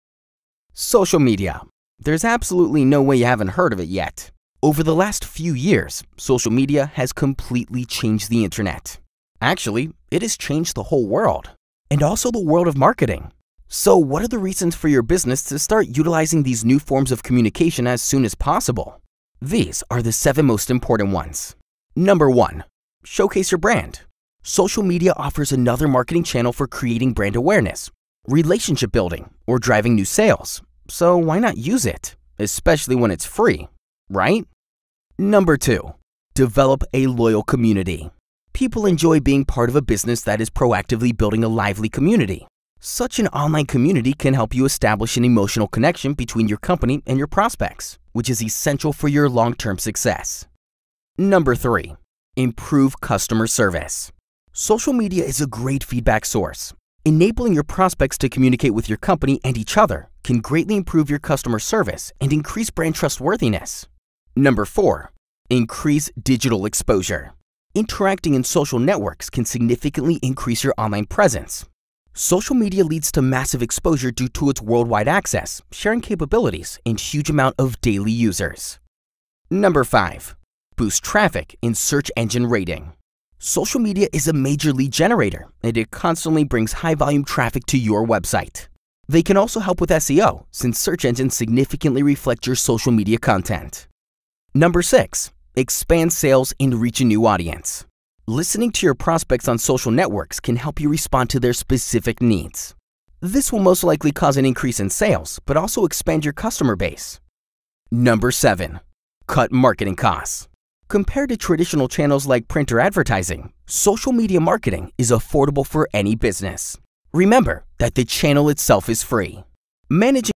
How Social Media Marketing Can Help Your Business online
A little video explaining how socialmedia works and how you can use it as part of your digitalmarketing strategy to grow your business.